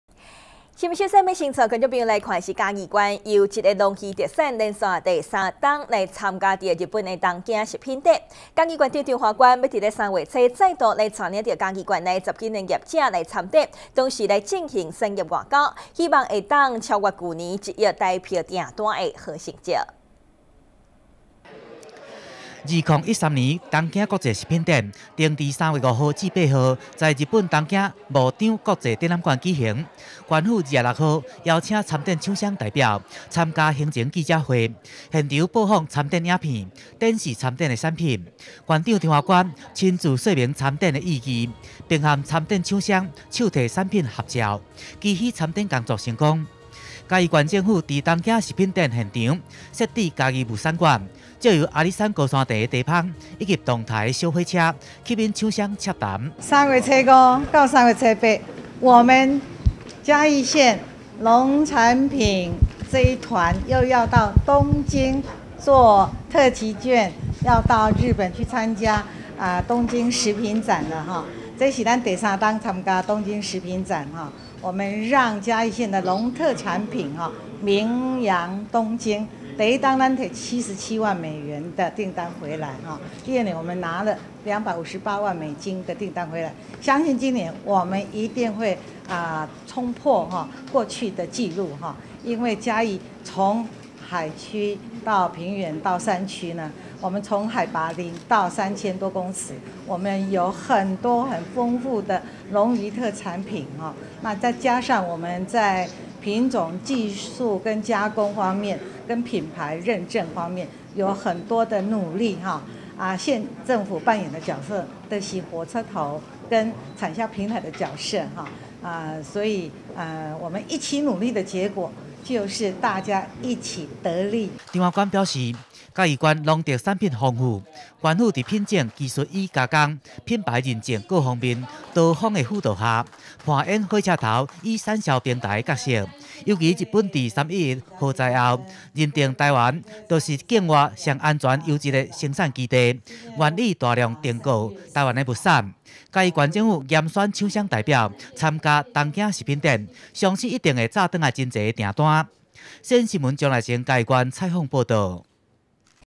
世新新聞 東京食品展 盼突破 1 億元訂單
東京食品展 盼突破 1 億元訂單

嘉義縣優質農漁特產連續3年參加日本東京食品展，縣長張花冠3月初將再度率領縣內10家業者參展，並進行產業外交，希望能超越去年1億台幣訂單的佳績。